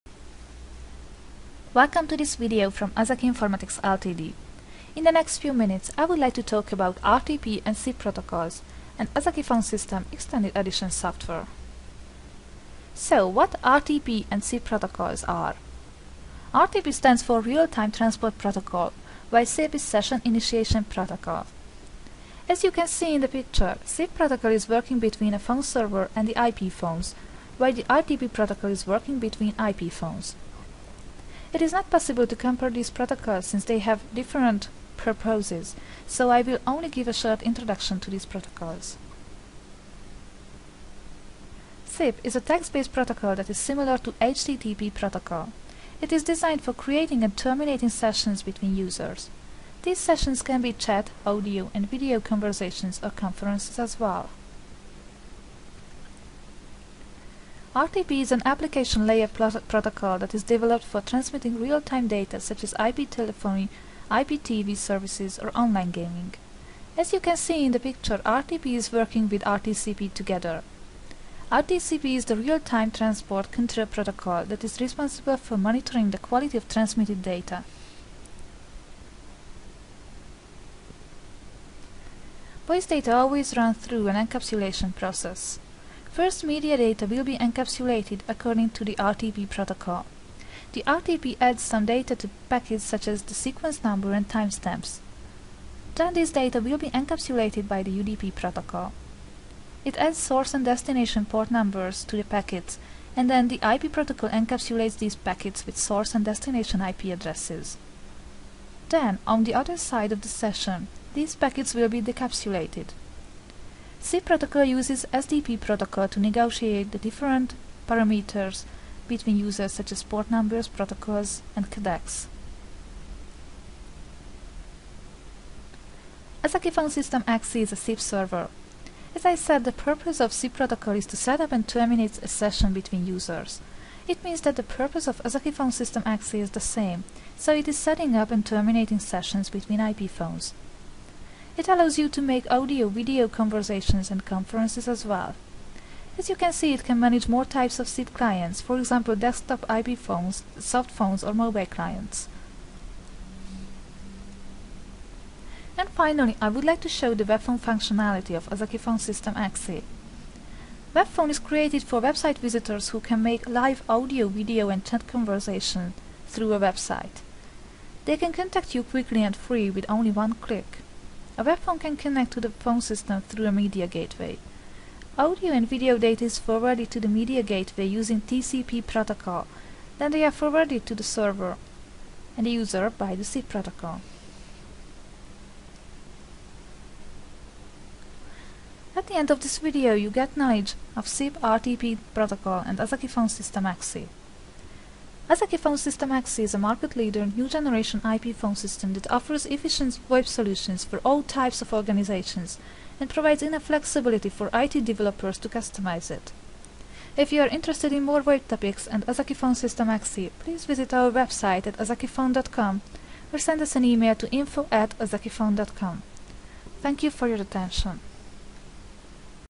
RTP vs SIP, a Modern Comparison of Realt-time Transport Protocol and Session Initiation Protocol
Real-time Transport Protocol is a peer-to-peer protocol that describes how video and audio data is transmitted through the Internet.

Session Initiation Protocol is an IP telephony signaling protocol. It establishes, modifies and also ends VoIP calls. Ozeki Phone System XE has a great support for VoIP technology that applies RTP and SIP for the communication